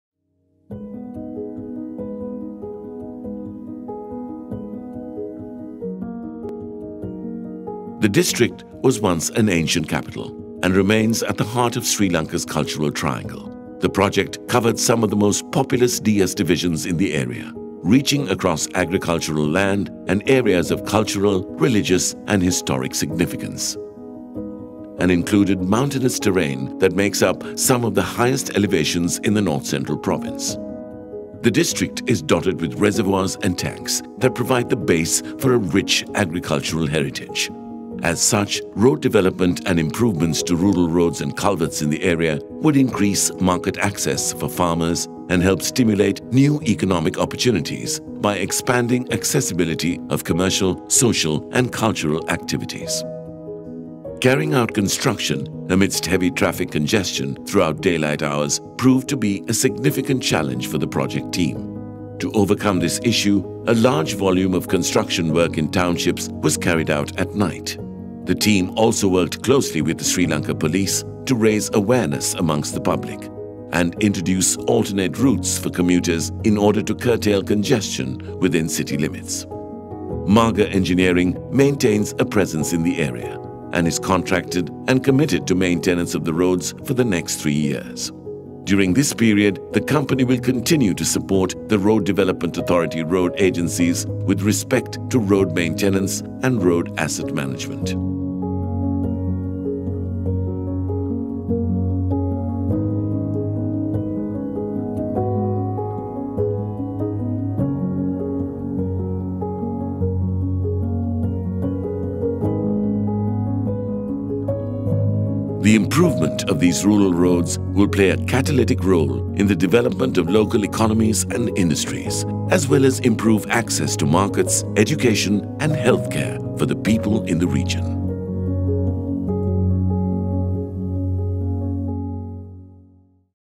Rehabilitation and improvement of Rural Roads in Anuradhapura
In 2019, MAGA completed 4 iRoad projects covering a road network of 332km in the Anuradhapura District at a total cost of LKR 6 billion. Rehabilitation and improvement of critical transport infrastructure in the area will improve mobility and increase access to Anuradhapura’s rich cultural heritage, while also stimulating economic development.